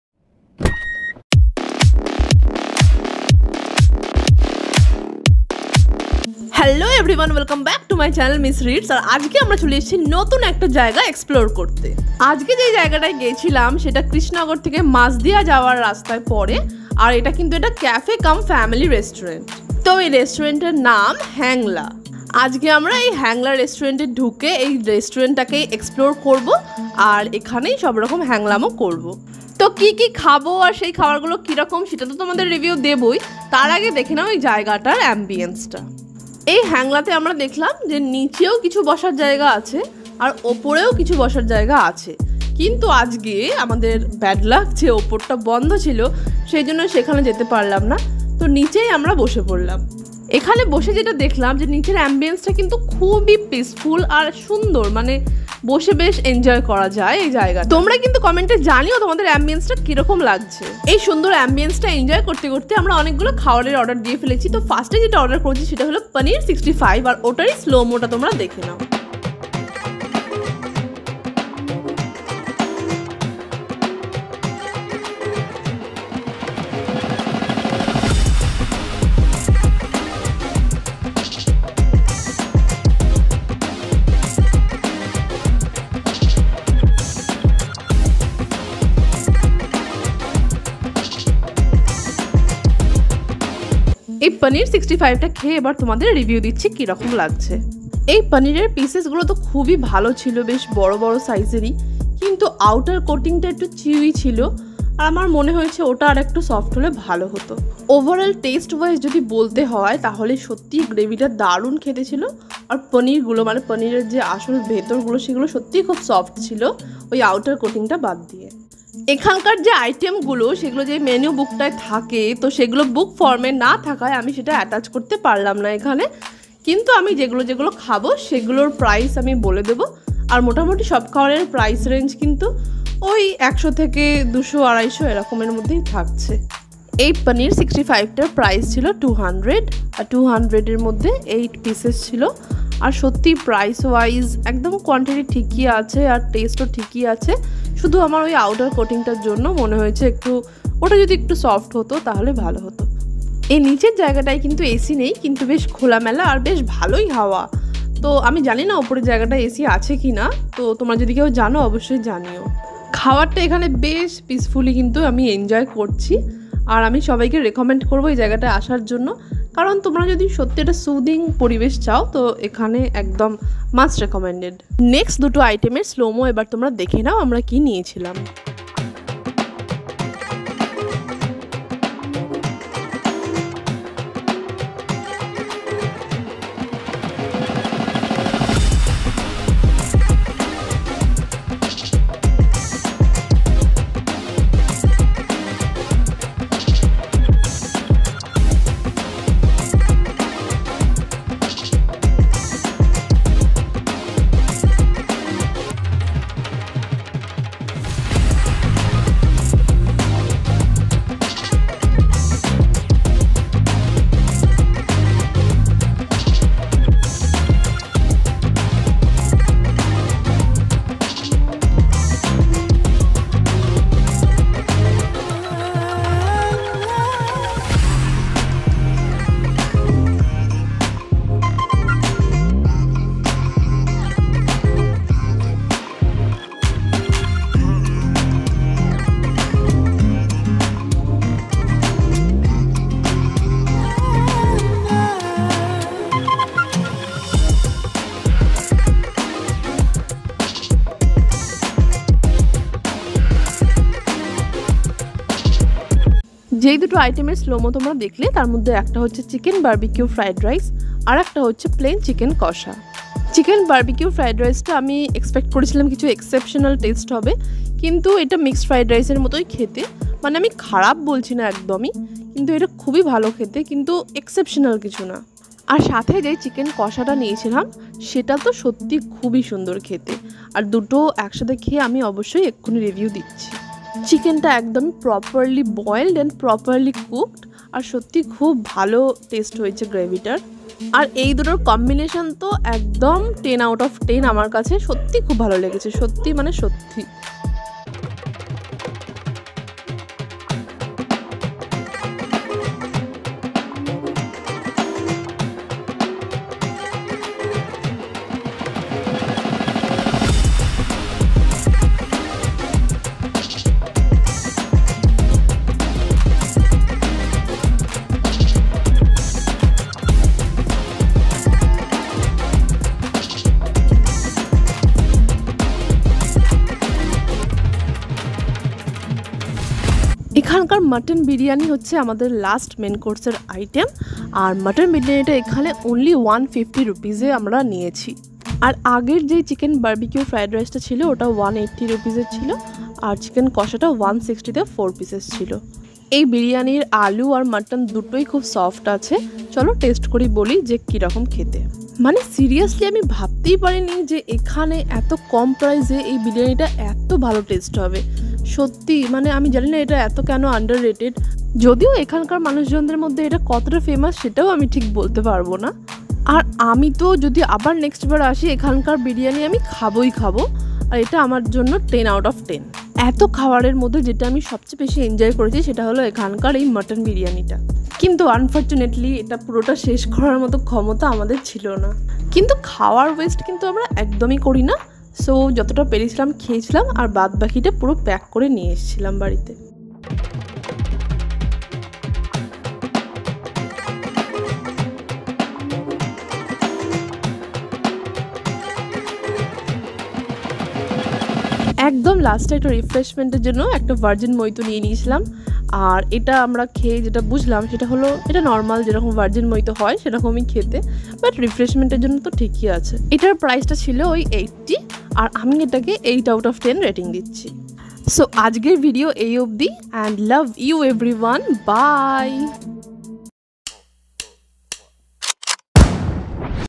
Hidden gem for foodies in Krishnagar | মাটন বিরিয়ানি খেলাম আজ অনেকদিন পরে 😋😋😋
Location : Hangla Cafe and restaurant (inbetween Krishnagar to Majdia road)